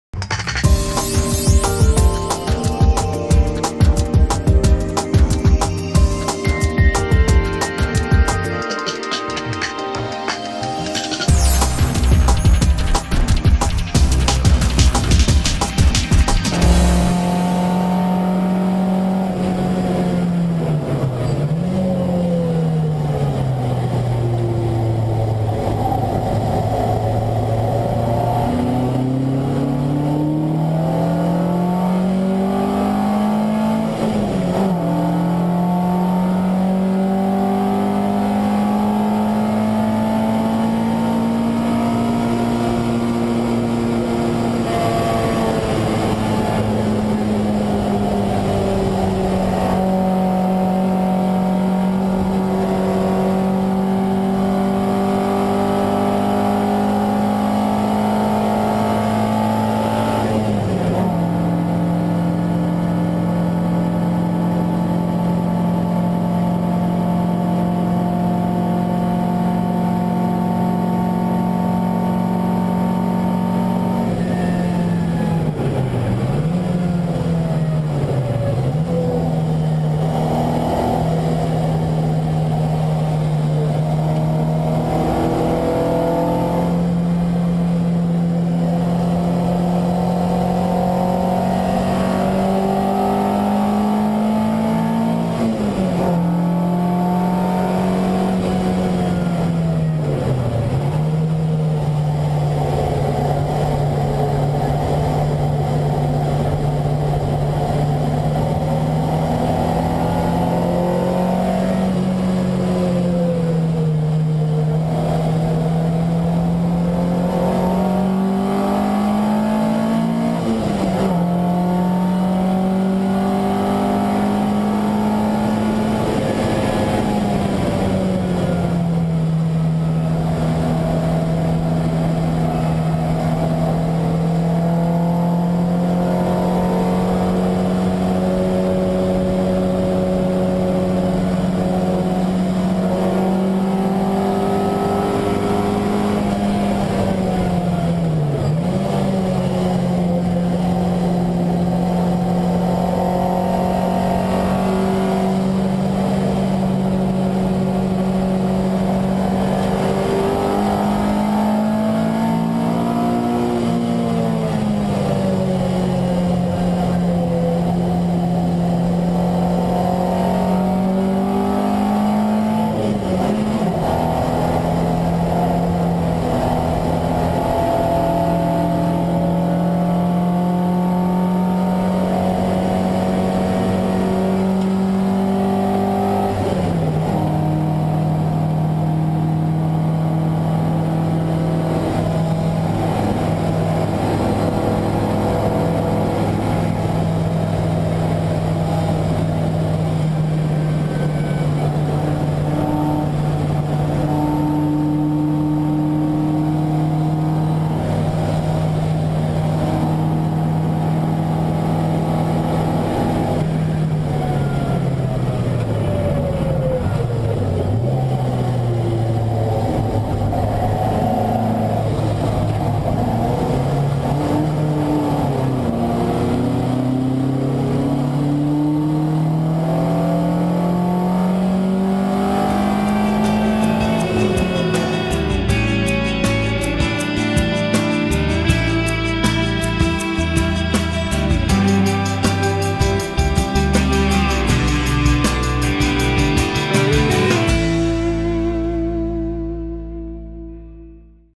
Spa Francorchamps 16.06.2014 Alfa Romeo Giulietta SV
Per Einarsson With data overlay from Harry's GPS Laptimer